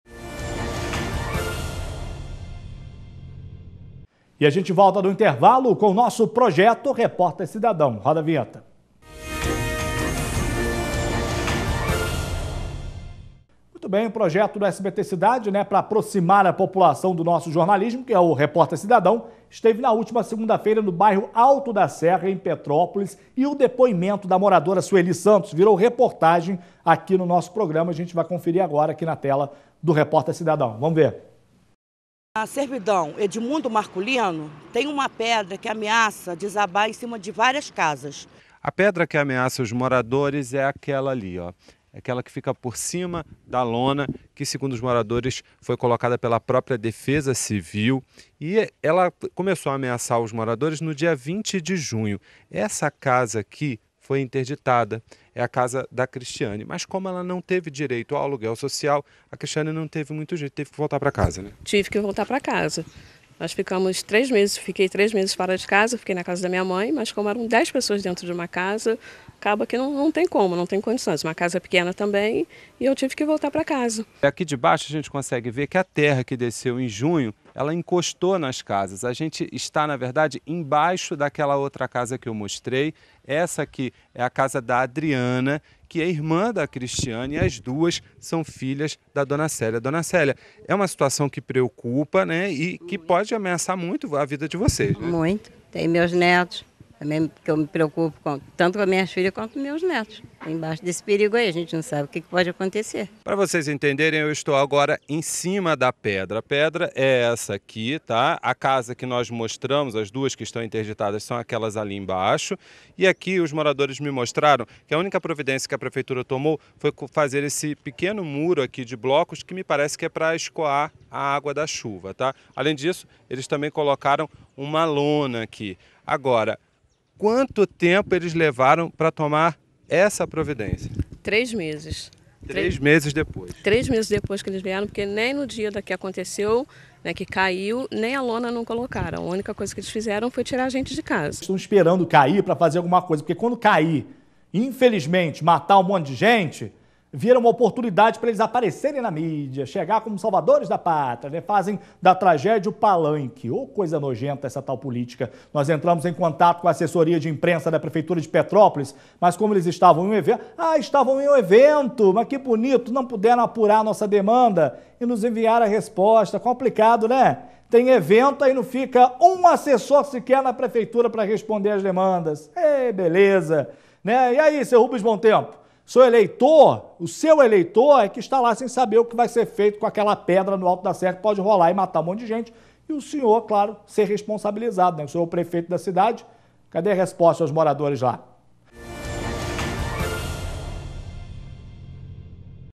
O Projeto Repórter Cidadão esteve na região serrana
Pedra ameaça moradores do morro dos Ferroviários no alto da serra.